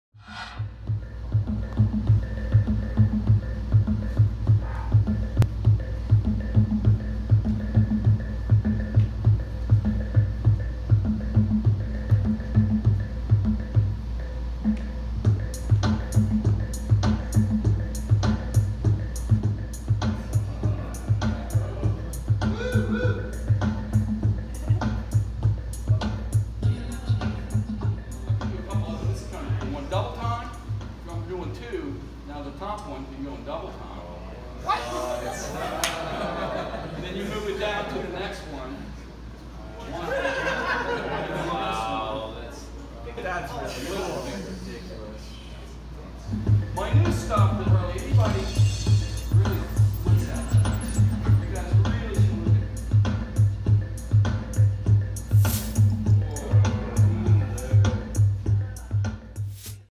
Confluence 2018 Finger Fitness workshop
Finger Fitness exercises directly address the physical needs of the hand by isolating and exercising natural movements.  Exercising your hands a few minutes a day can improve hand strength, limberness and coordination. Great for musicians, surgeons, magicians, athletes,  gamers, and so many more. Highly recommended for the young and old to keep your hands fit for life. Take care of your hands and they will take care of you!